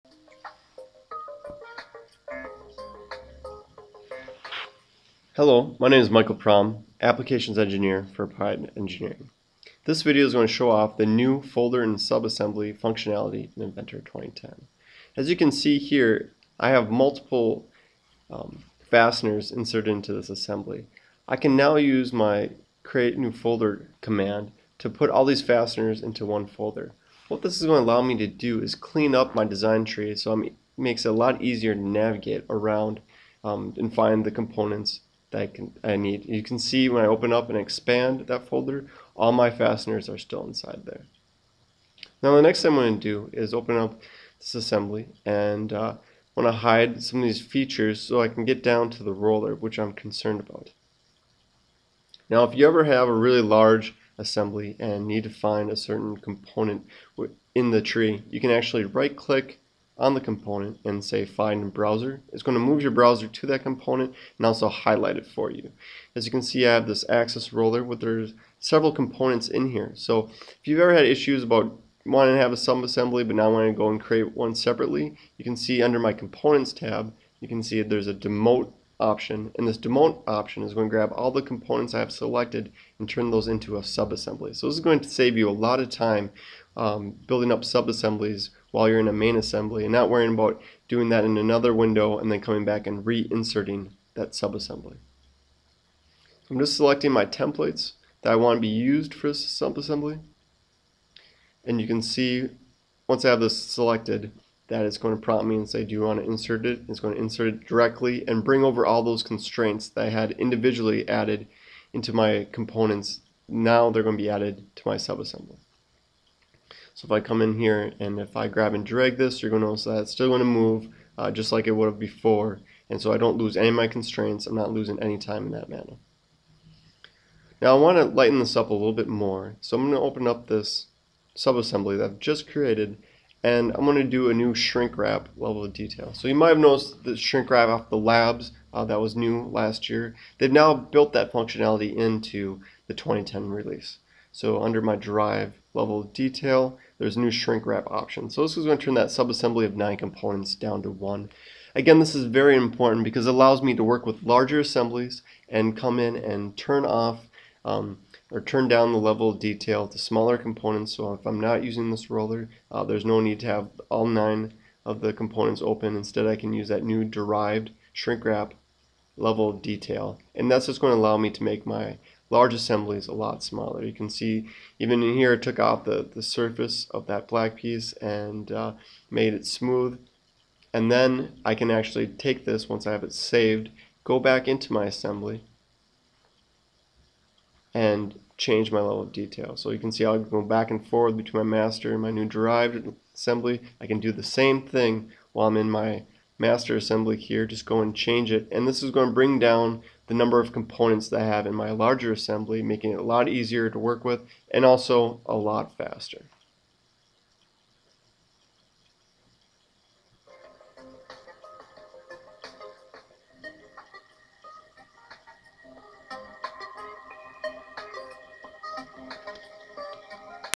What's New Inventor 2010 Folder & Sub Assm
Mike Prom, Application Engineer at Applied Engineering, covers the ability to organize your design tree using folders. Also, Mike shows how to create sub-assemblies inside of assemblies in Autodesk Inventor 2010. Best viewed in HD in fullscreen mode.